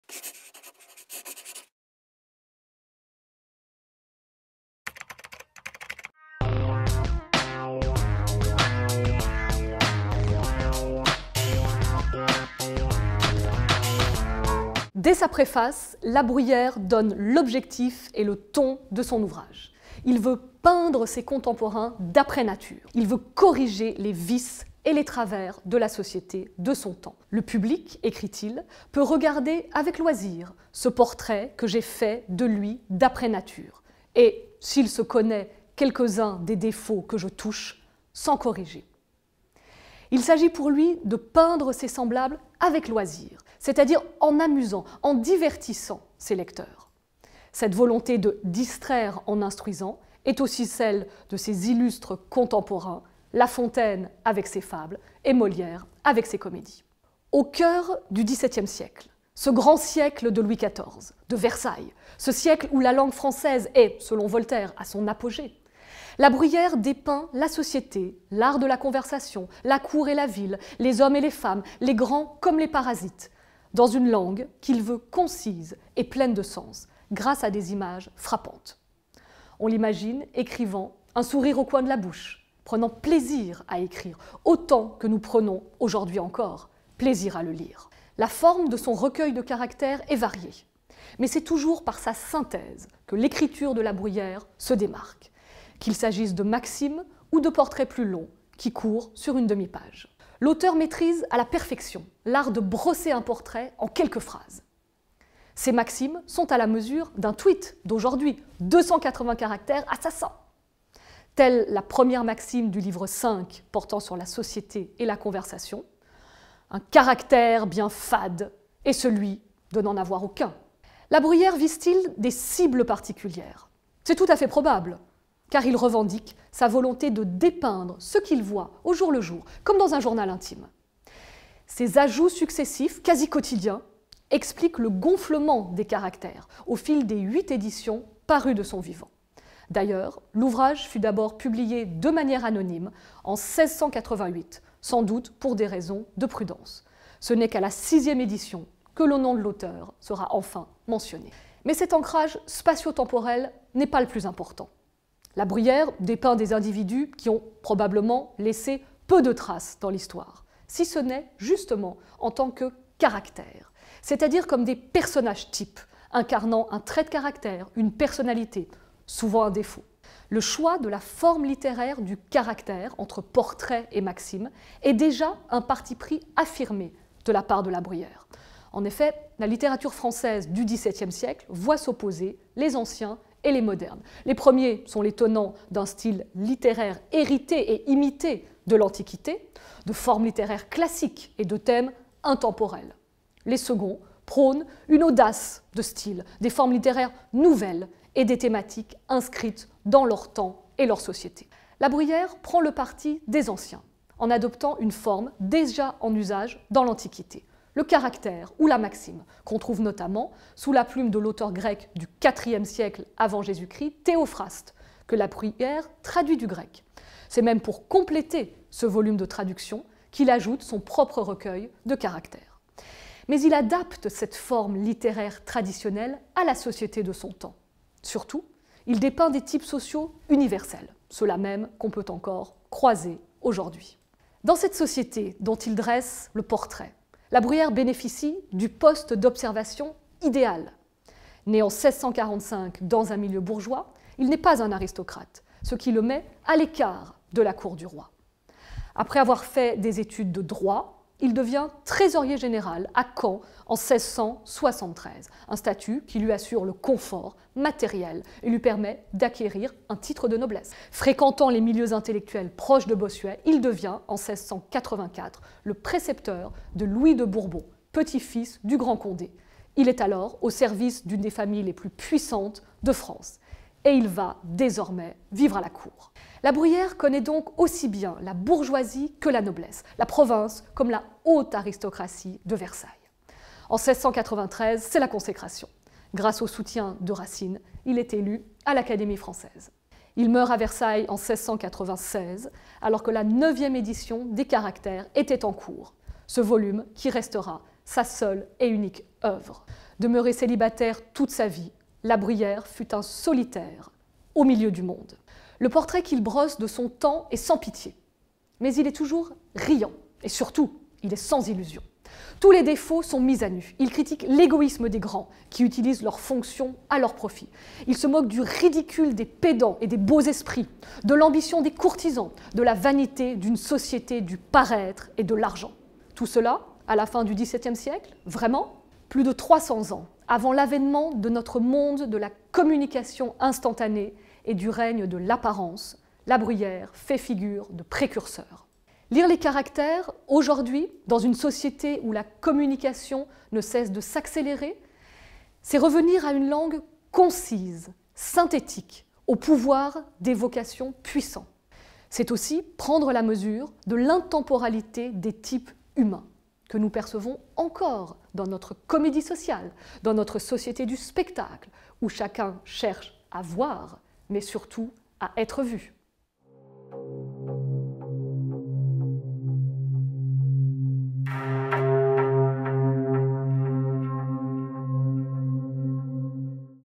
Blanche c'est pas classique - Jean La Bruyère "Les caractères"
Blanche Cerquiglini vous présente l'ouvrage "Les caractères, livres V à X : programme du bac" de Jean La Bruyère aux éditions Gallimard.